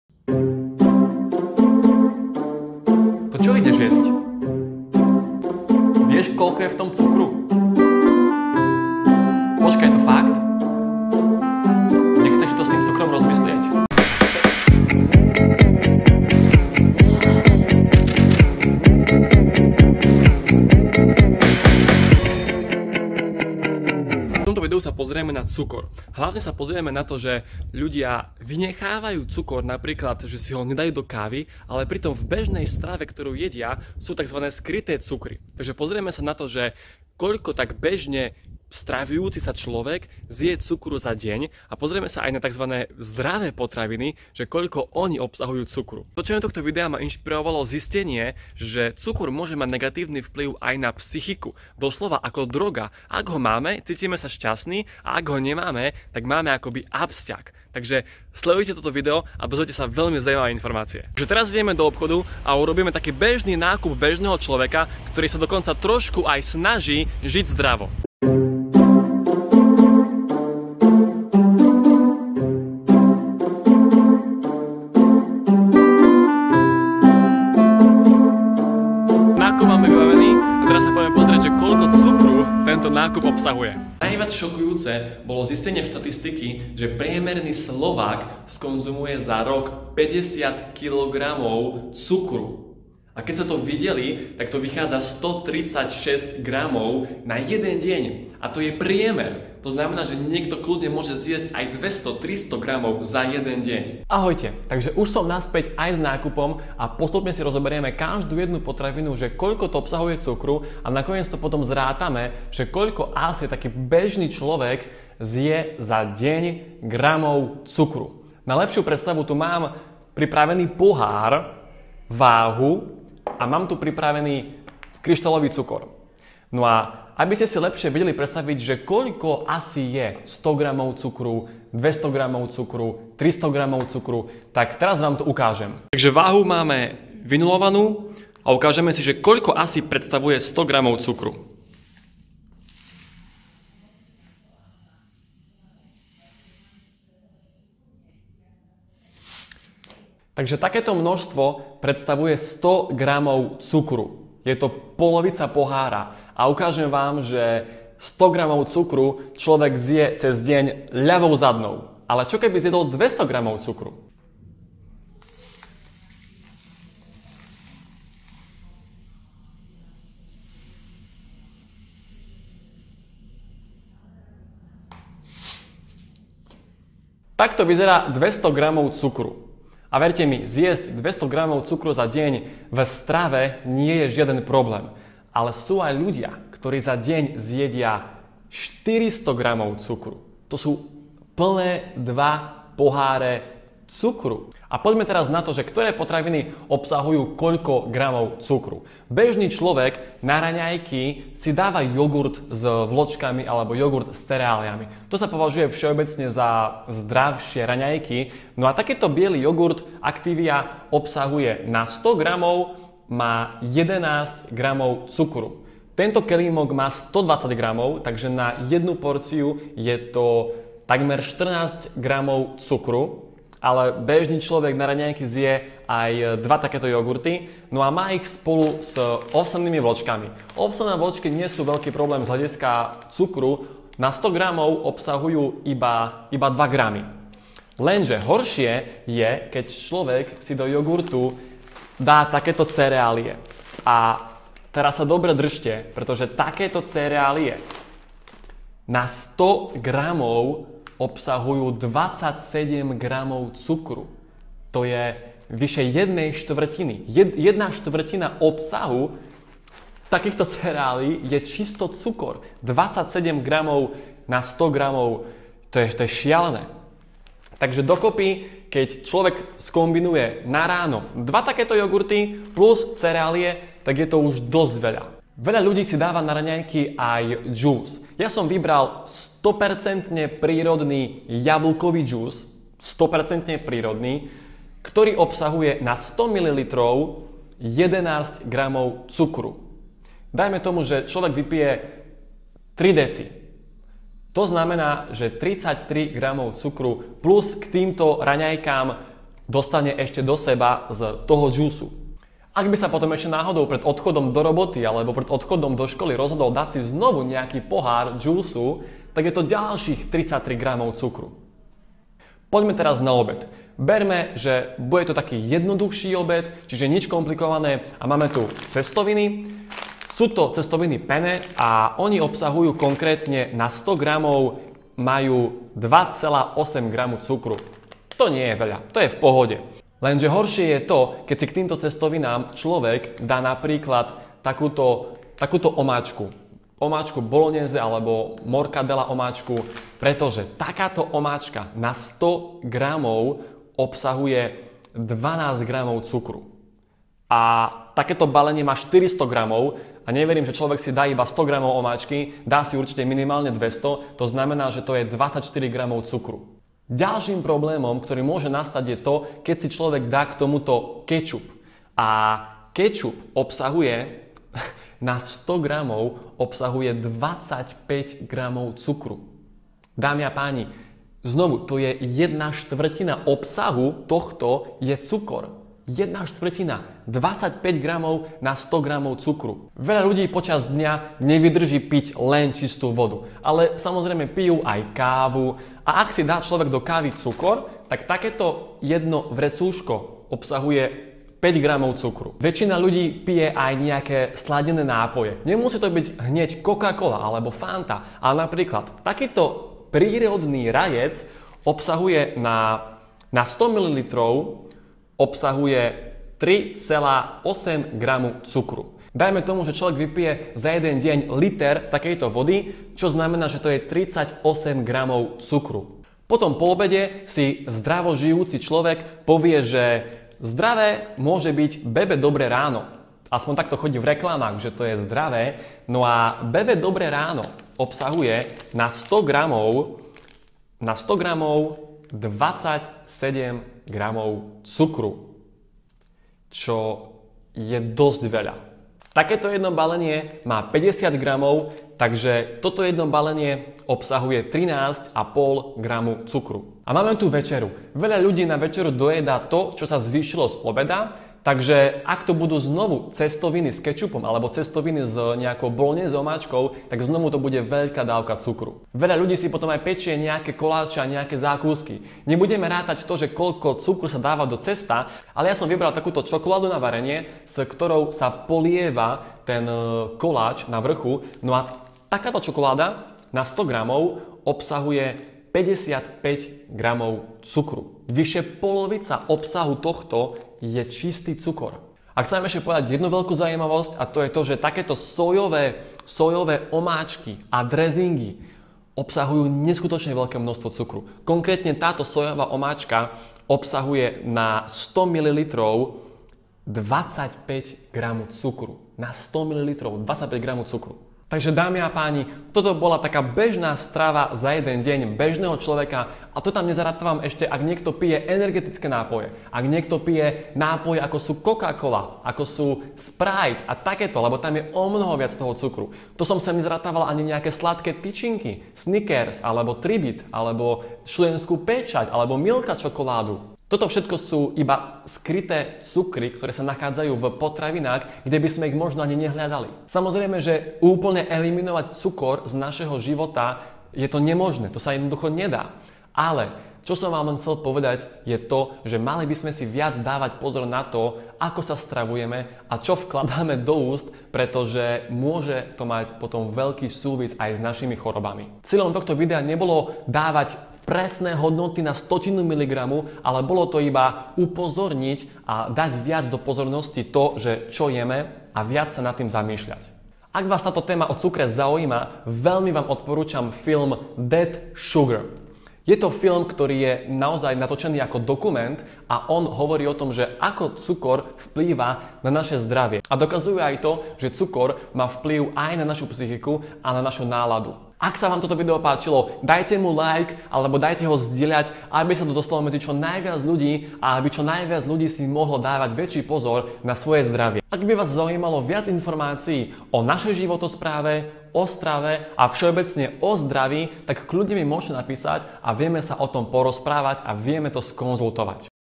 Zješ 2 plné poháre cukru denne ?
Cieľom tohto videa nie je podávať presné informácie na stotinu gramu. Cieľom je Vám ukázať to, že aj "zdravé" potraviny, obsahujú skryté cukry a mali by sme sa na zloženie pozerať častejšie. Podľa môjho názoru, by v našej strave mala prevažovať surová potraviny v prirodzenej podobe.

Budem rád ak toto video budete zdieľať.